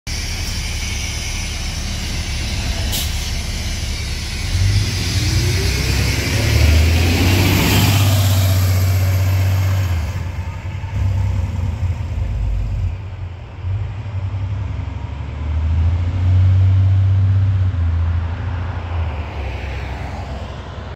Ex AVTA 2008 MCI D4500CT #4750
Spotted at Lincoln Heights
Engine: Cummins ISM
Transmission: Allison B500

Operated by LA Charter Bus Lines

Flickr - SoCal Transit Studios
CPTDB - Newflyerxn40